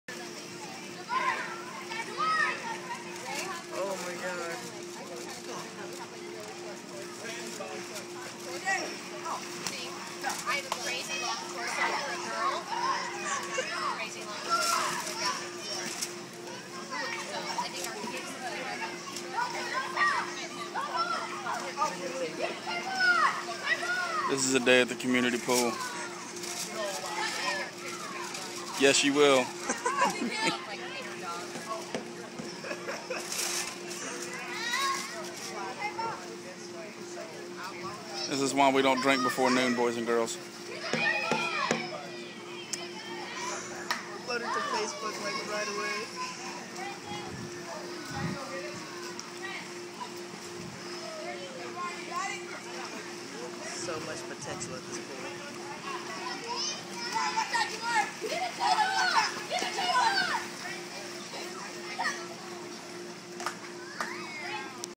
Why we don't drink before noon!
This was at my housing complex community pool taken 5/09/10@ 1pm. This dude did this for at least 2 hours. NOTICE he's not in the sun to get any suntan he's in shade and listening to an  MP3 player. I will go back over the next few weekends and try to catch him again!